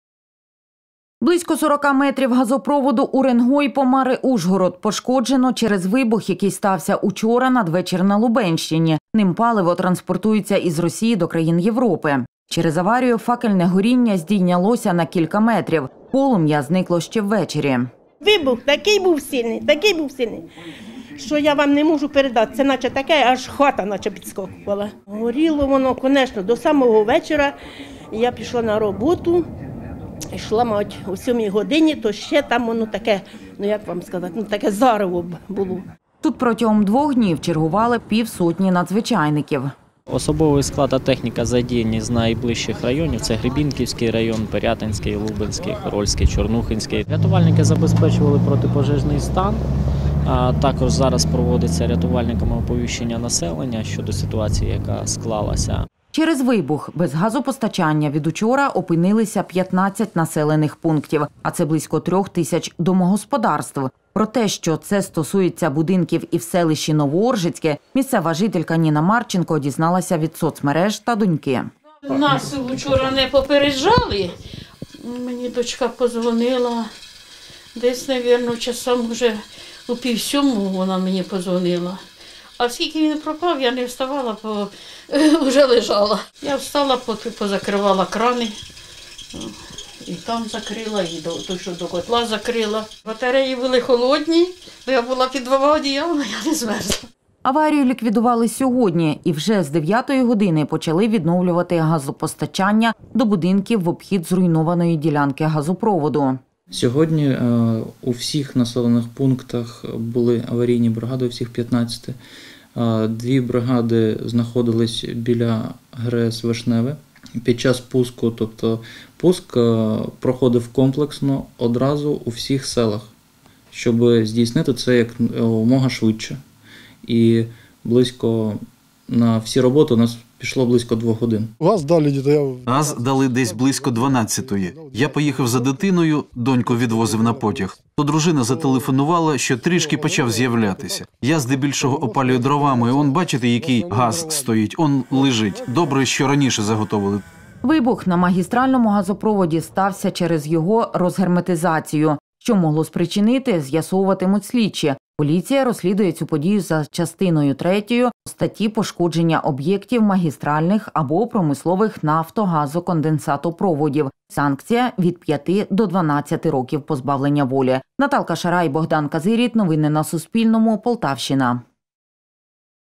До 15 населених пунктів Полтавщини відновили постачання після аварії на газопроводі
Менше доби були без газу будинки на Полтавщині, які залишилися без постачання через вибух на газопроводі. Його почали відновлювати сьогодні зранку. Про це розповів Суспільному заступник гендиректора підприємства «Лубнигаз» Єгор Скурідін. Саме ж факельне горіння зупинилося ще вчора. А відсьогодні тампочали ремонтувати магістральний газопровід.